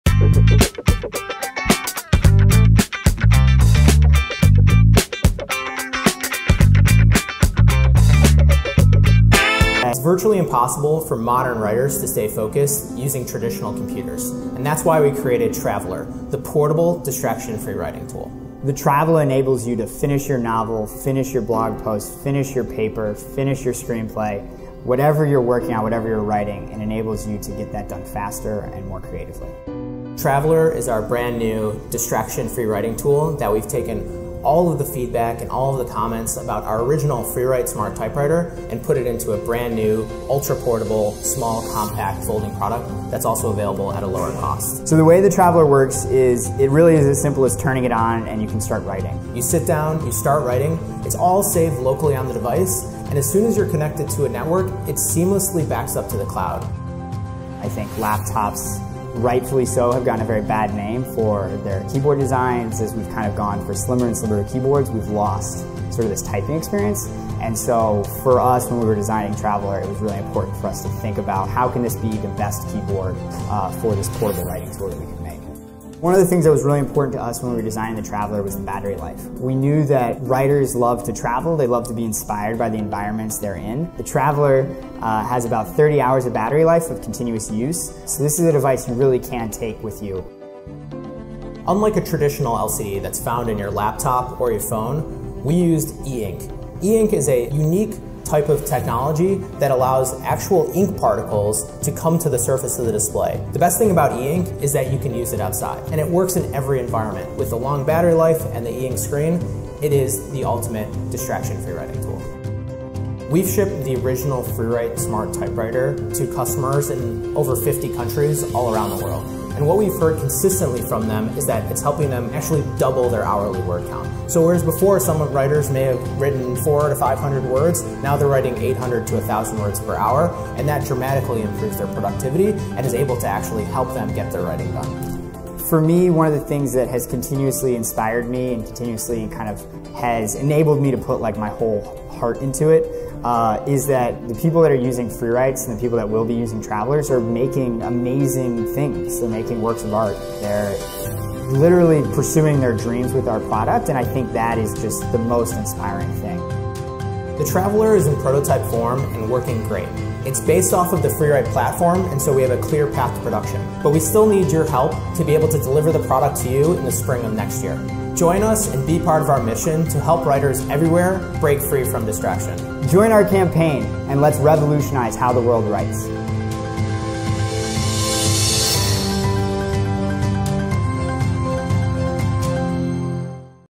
Free Write - Traveler printing device for your thoughts at any time
Say goodbye to writer's block! Traveler is the only device designed to help you focus on writing. Whether it's your novel, screenplay, or blog, write wherever you go.